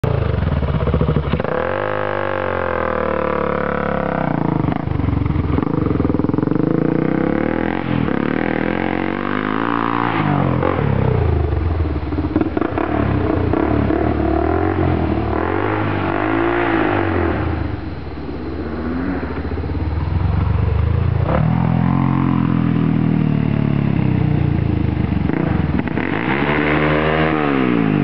2001 Honda XR400 Hill Climb at Mines and Meadows
Frank ascends then descends a large hill at Mines and Meadows ATV Resort in Wampum, PA on 8/27/11. Cousin Mario looks on with his 2008 Honda 150R.